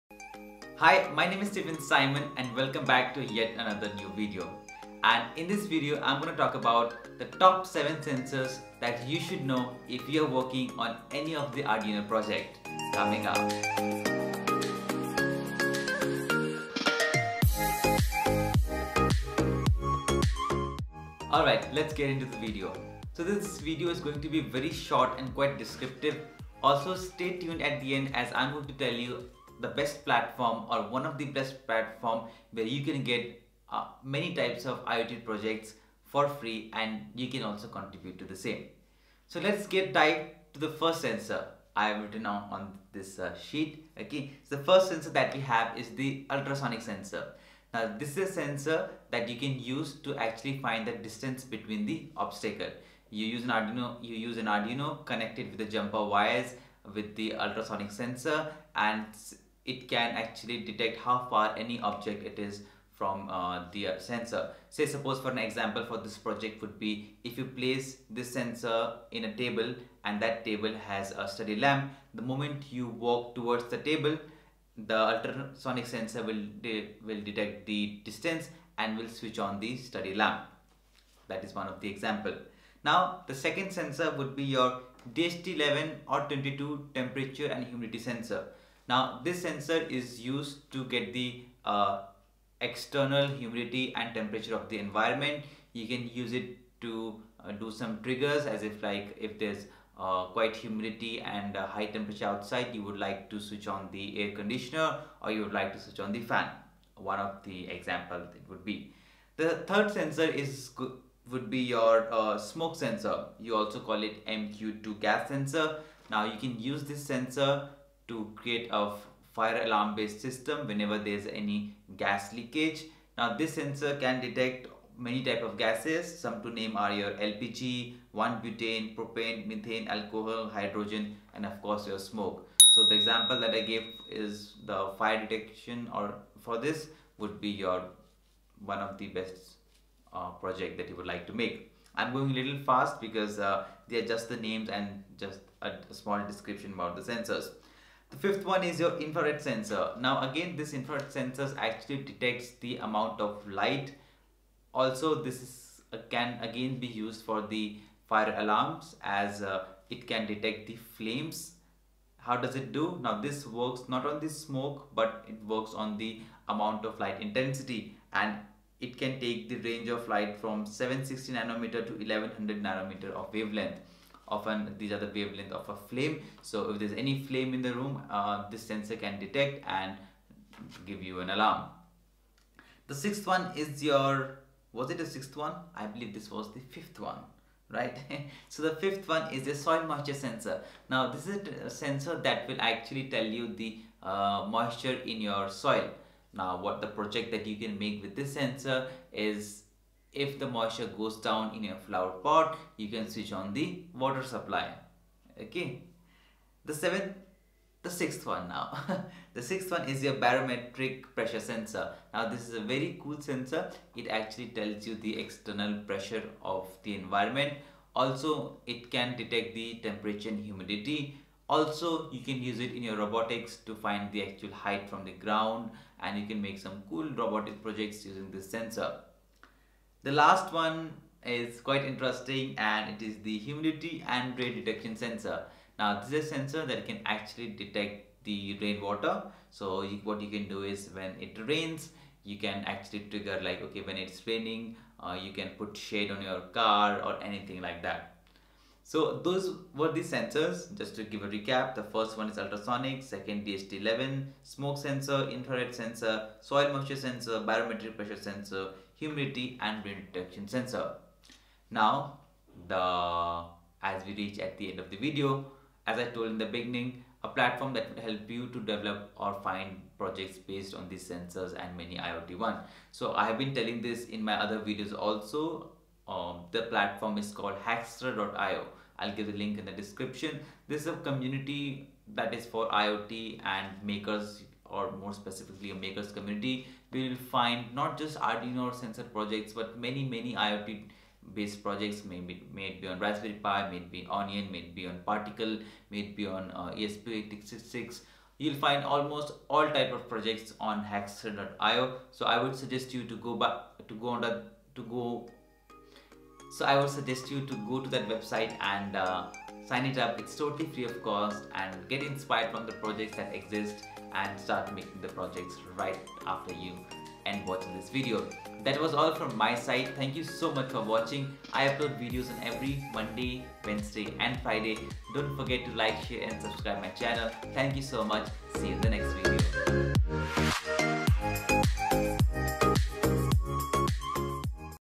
7 Arduino Sensors you must know | Top Arduino Sensor || Stephen Simon
This videos cover top seven sensors you must know if you are making project on Arduino!